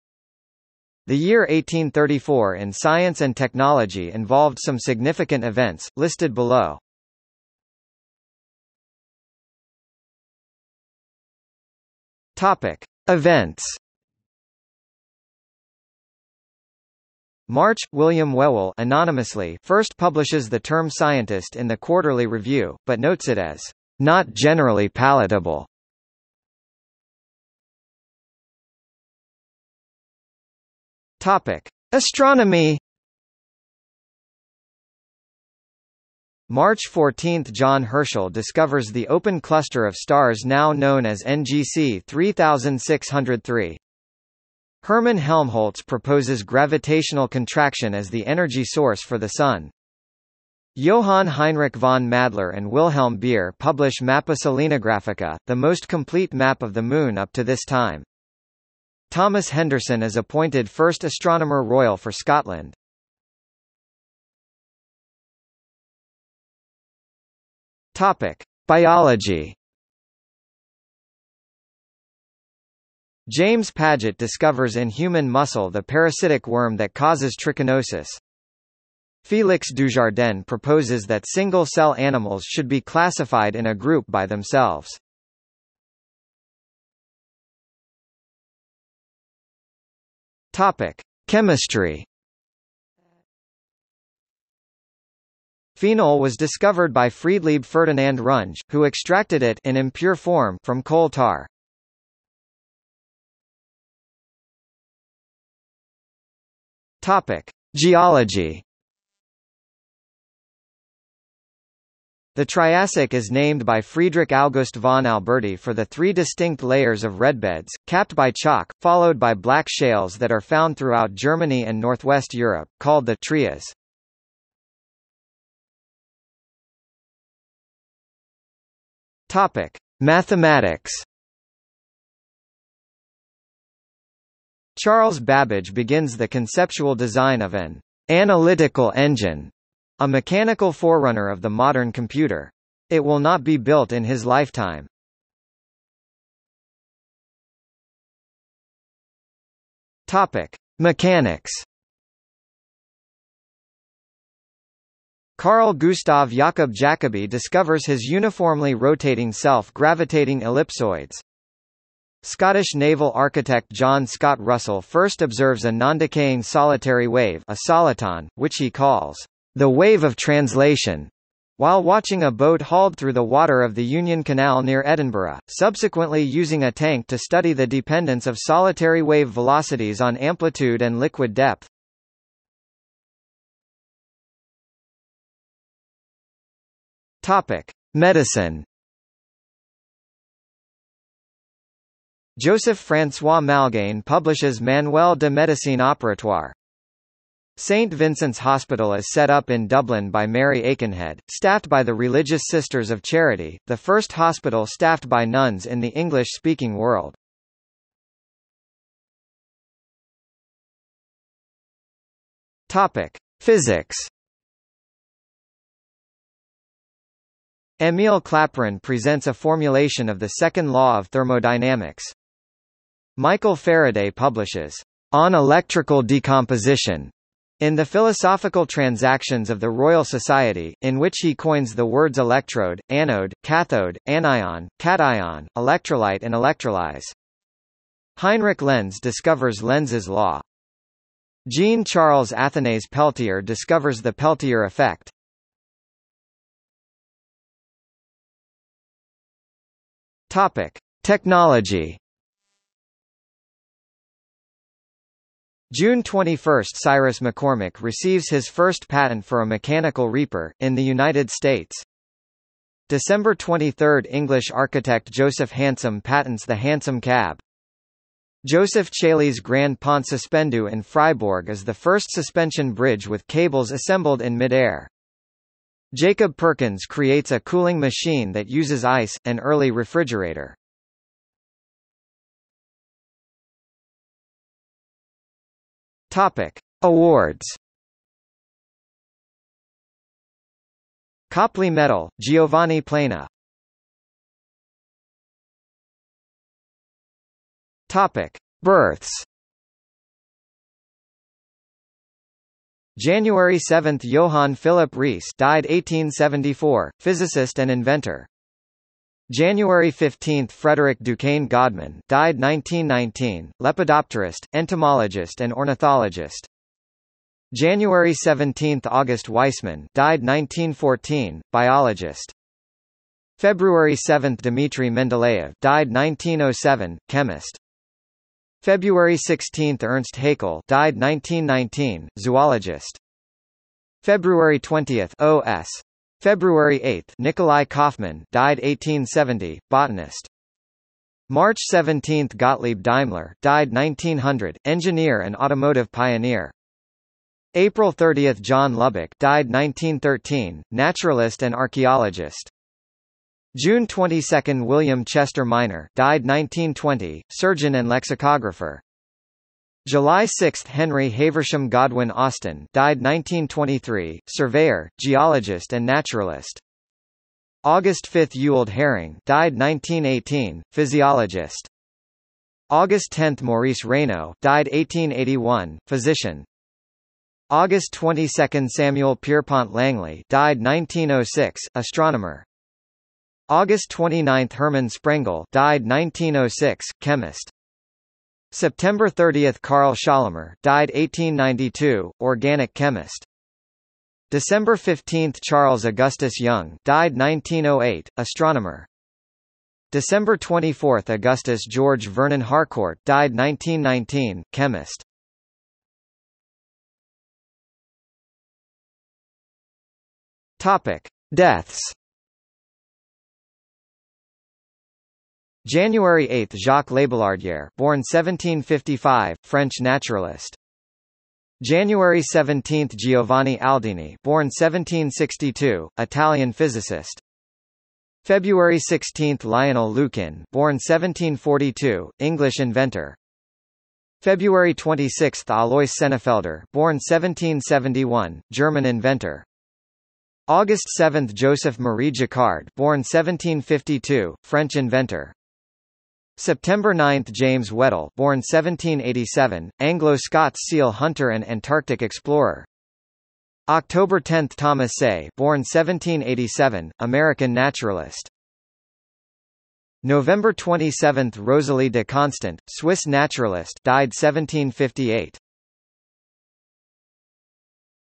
1834 in science | Wikipedia audio article
00:00:08 1 Events
00:00:27 2 Astronomy
00:01:04 3 Biology
00:01:26 4 Chemistry
00:01:43 5 Geology
00:02:05 6 Mathematics
00:02:26 7 Mechanics
00:03:03 8 Medicine
00:03:29 9 Physics
00:04:06 10 Technology
00:04:42 11 Awards
00:04:54 12 Births
00:06:46 13 Deaths

- Socrates